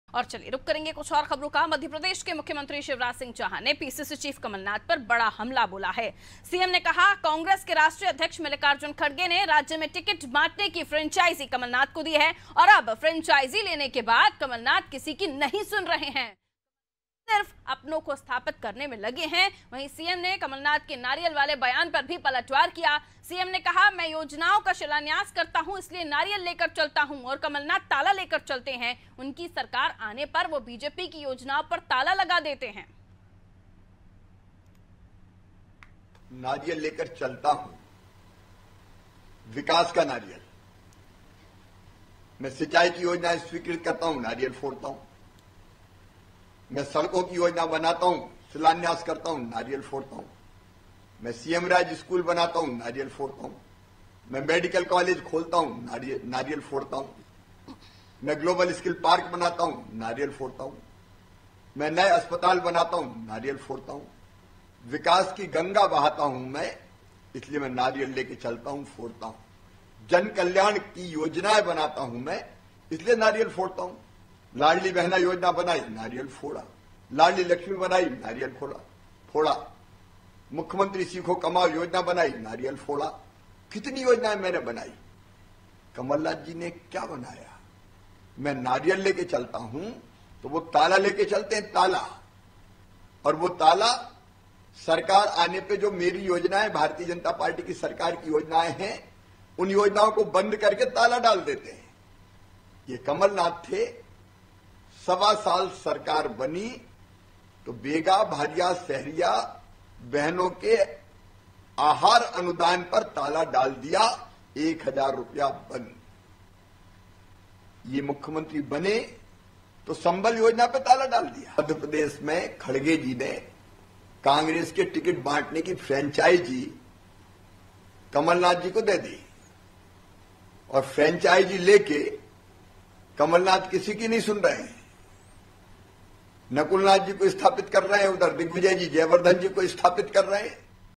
Bhopal : सीएम शिवराज का कमलनाथ पर बड़ा हमला, कमलनाथ को मिली टिकट बांटने की फ्रेंचाइजी || Anaadi Tv
मध्य प्रदेश के मुख्यमंत्री शिवराज सिंह चौहान ने पीसीसी चीफ कमलनाथ पर बड़ा हमला बोला है... सीएम ने कहा कि... कांग्रेस के राष्ट्रीय अध्यक्ष मल्लिकार्जुन खडगे ने राज्य में टिकट बांटने की फ्रेंचाइजी कमलनाथ को दी है... और अब फ्रेंचाइजी लेने के बाद कमलनाथ किसी की नहीं सुन रहे हैं... कमलनाथ सिर्फ अपनों को स्थापित करने में लगे हैं... वहीं सीएम ने कमलनाथ के नारियल वाले बयान पर भी पलटवार किया...सीएम ने कहा कि... मैं योजनाओं का शिलान्यास करता हूं...इसीलिए नारियल लेकर चलता हूं...और कमलनाथ ताला लेकर चलते हैं... उनकी सरकार आने पर वे बीजेपी की योजनाओं पर ताला लगा देते हैं...

COMING SOON
 DEN, AIRTEL